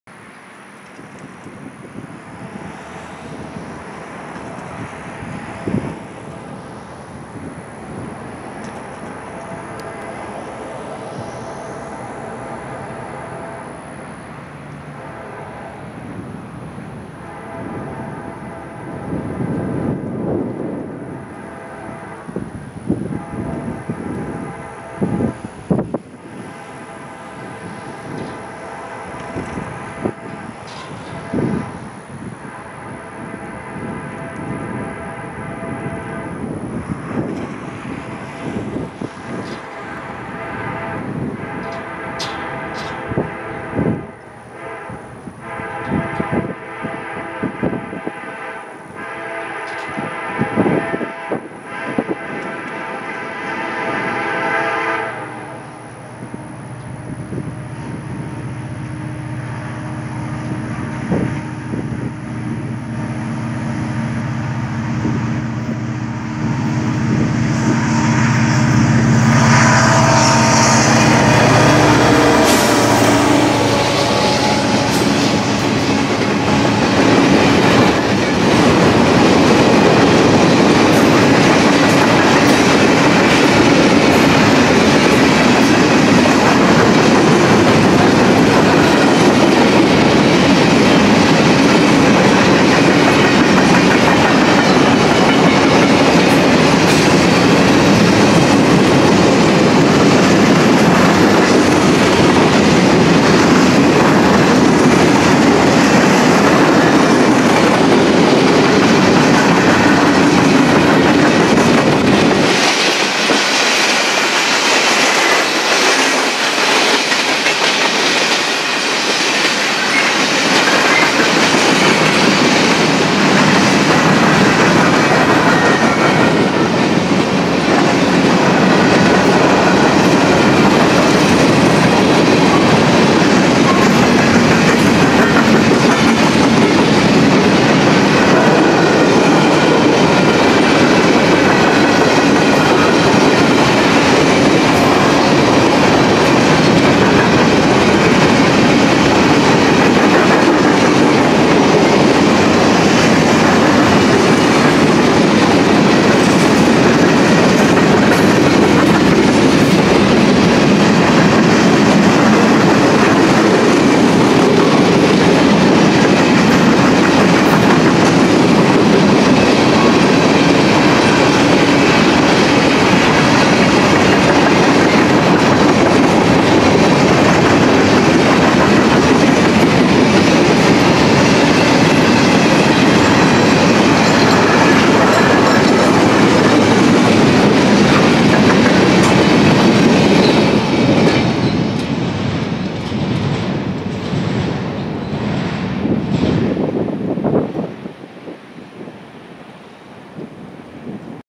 CN Empty Grain North through Hammond, LA
Usually on Fridays I am free, so why not round out a week with a little bit of railfanning? After arriving at the Hammond, LA station right after AMTK P058 departed, the signal was already in the process of changing to green once again. After not much waiting, the signal was green again, and a headlight appeared to the South. Here we see an empty grain headed North through the city of Hammond, LA with a CN SD70M-2 and a ES44DC on the head end. Taken on 3/18/22.